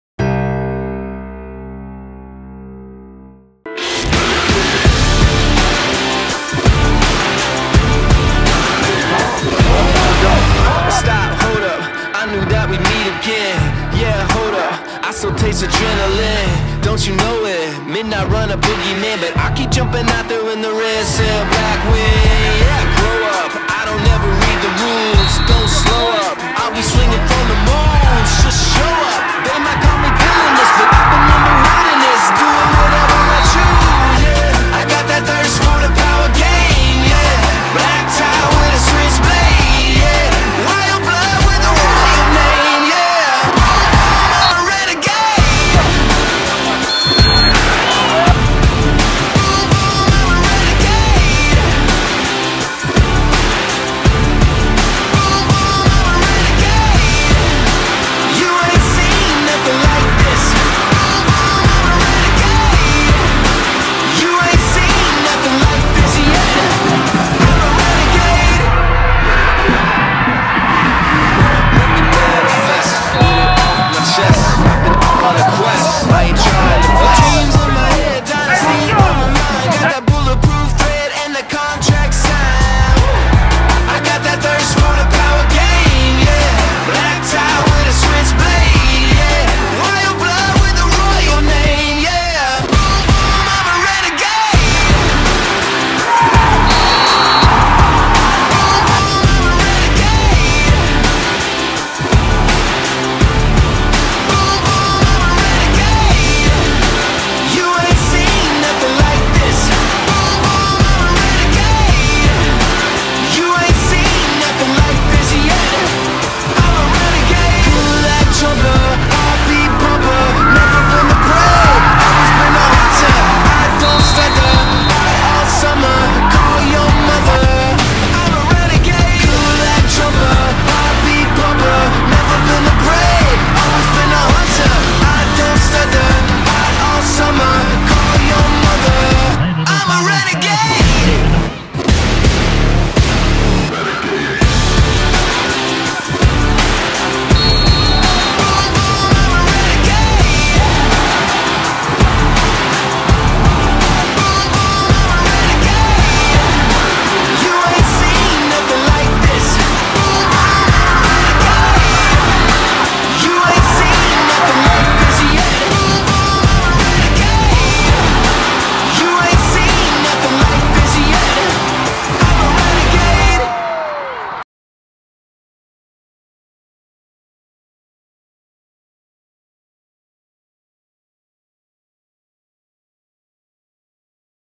New Albany VS Pickerington North | Football Highlights
New Albany High School VS Pickerington North High School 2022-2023 Football

All content is property of iHP Sports, and cannot be reproduced, re-edited or re-distributed in any form without the express written consent iHP Sports.

Follow @itsihunter and @ihpsportsmedia on other social media platforms too